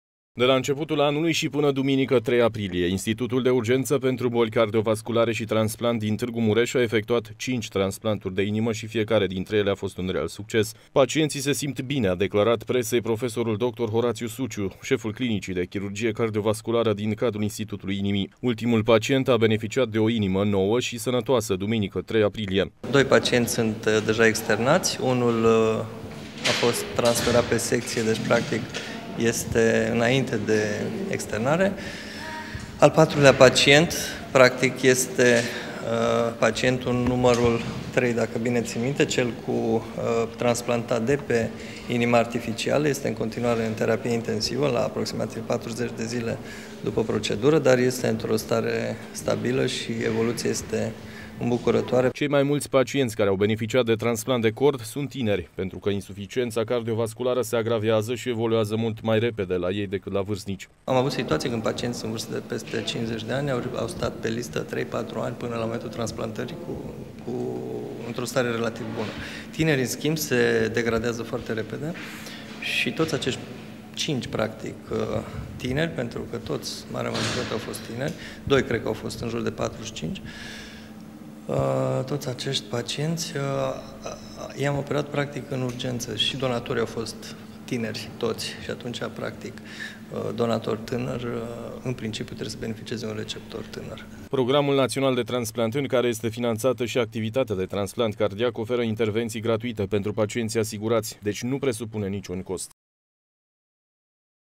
RECORD DE TRANSPLANTURI CARDIACE LA IuBCvT
În medie, Institutul pentru Boli Cardiovasculare şi Tranplant din Tîrgu Mureş, efectuează cel mult opt transplanturi de cord. Anul acesta, până acum Institutul Inimii a reuşit să salveze deja cinci vieţi, prin transplant de inimă.